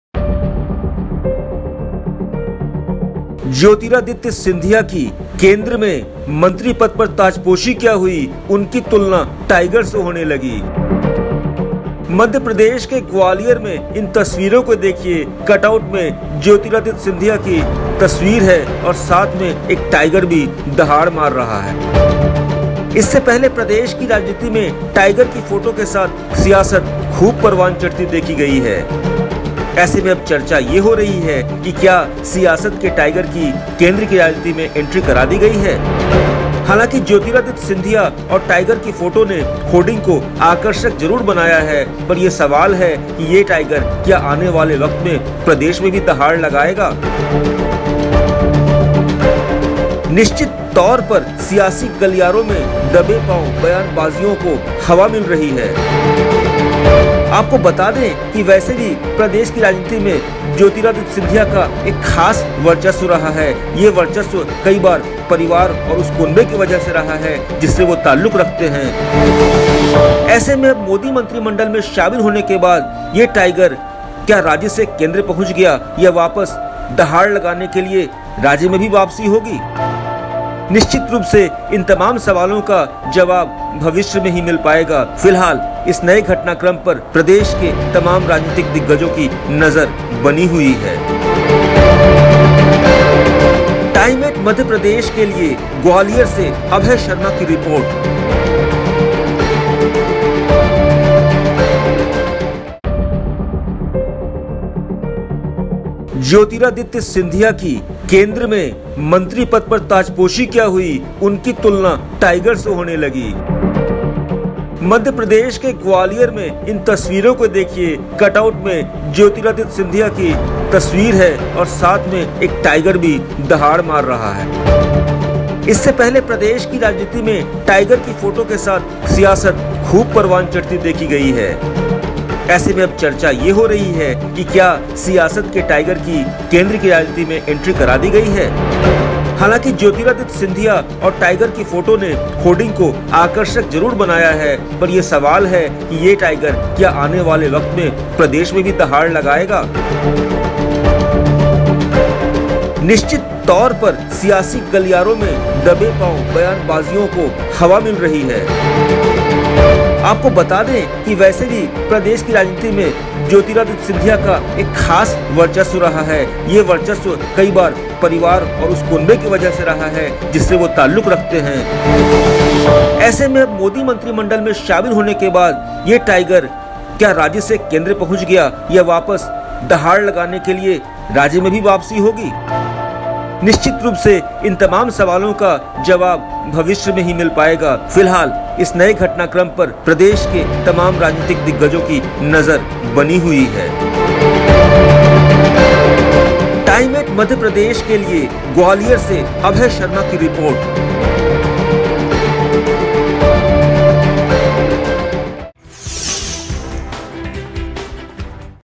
TIME8 SPECIAL REPORT I ज्योतिरादित्य की ताजपोशी के साथ फिर शुरु हुआ ‘टाइगर’ एपिसोड
ग्वालियर I केंद्र में मंत्री पद पर काबिज़ होने के बाद मप्र की सियासत में एक बार फिर ‘टाइगर’ एपिसोड शरु हो गया है। शपथ लेने के बाद ग्वालियर के चौक चौराहे ‘टाइगर इस बैक’ के होर्डिंग्स से पट गए हैं।